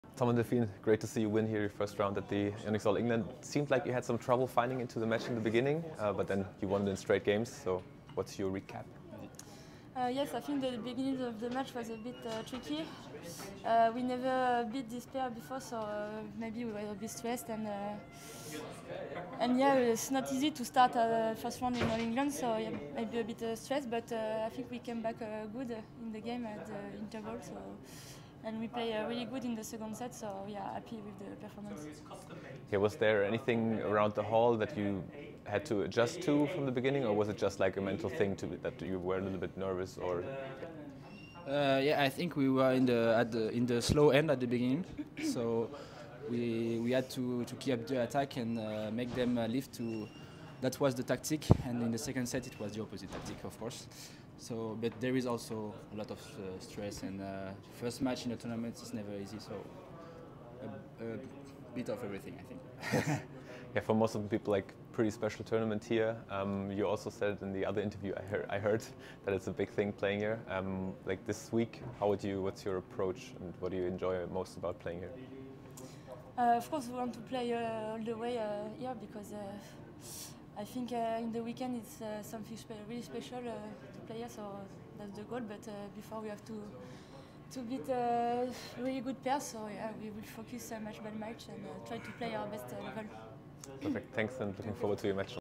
YONEX All England 2023: Thom Gicquel and Delphine Delrue win their opener
Thom Gicquel and Delphine Delrue have made it to the Round of 16 at the YONEX All England 2023. In their opener the French beat Ko/Eom from Korea 21-19, 21-15.